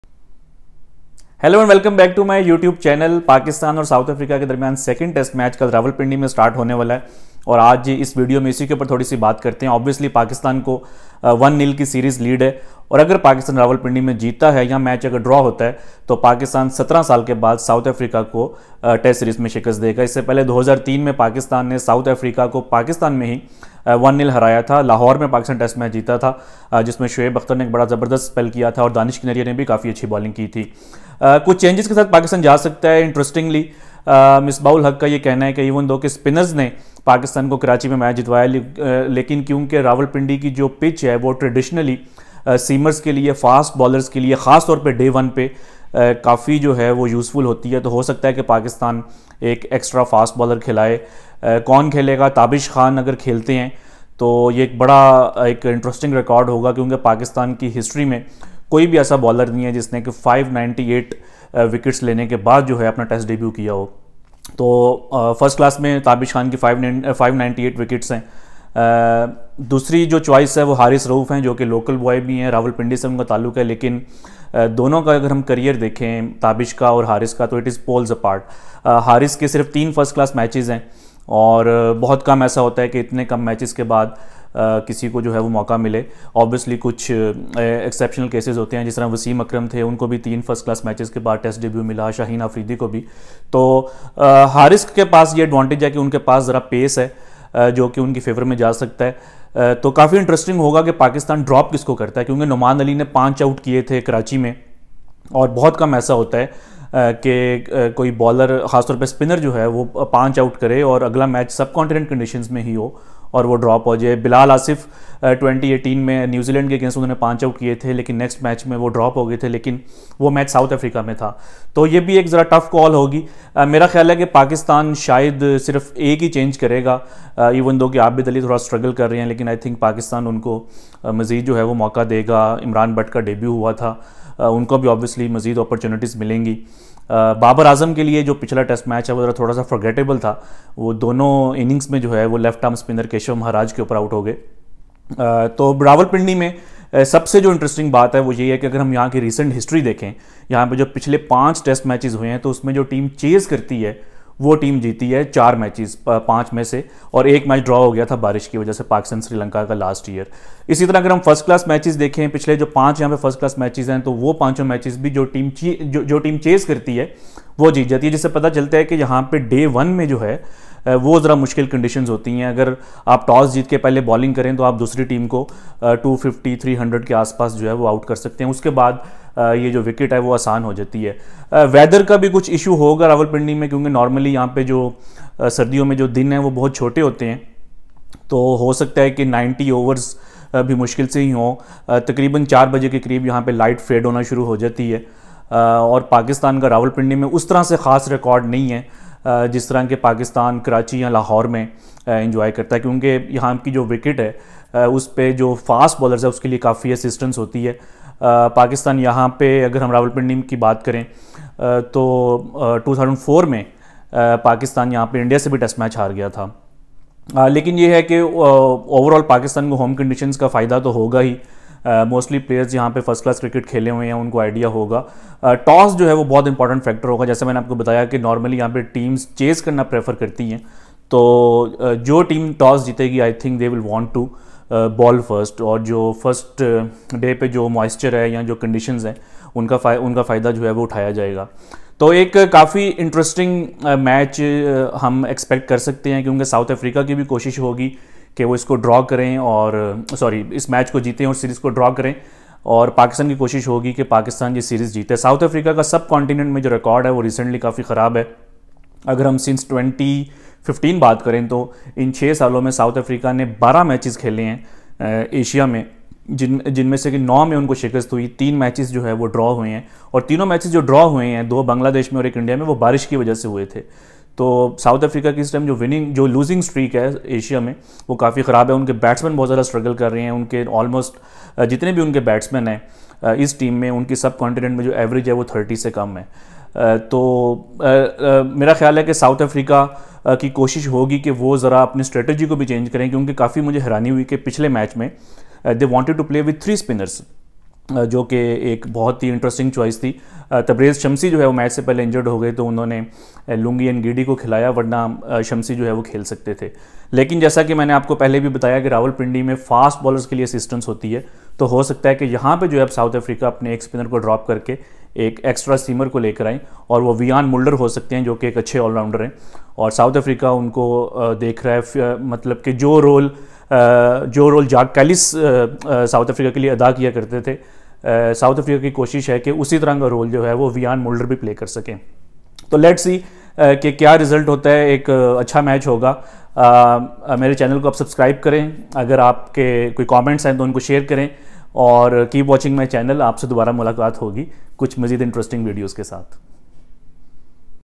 Pakistan eyeing first series win vs South Africa in 17 years | 2nd Test Preview
Pakistan might go with an extra seamer in the second Test which means a debut for Tabish Khan or Haris Rauf. Here are some thoughts ahead of the second Test.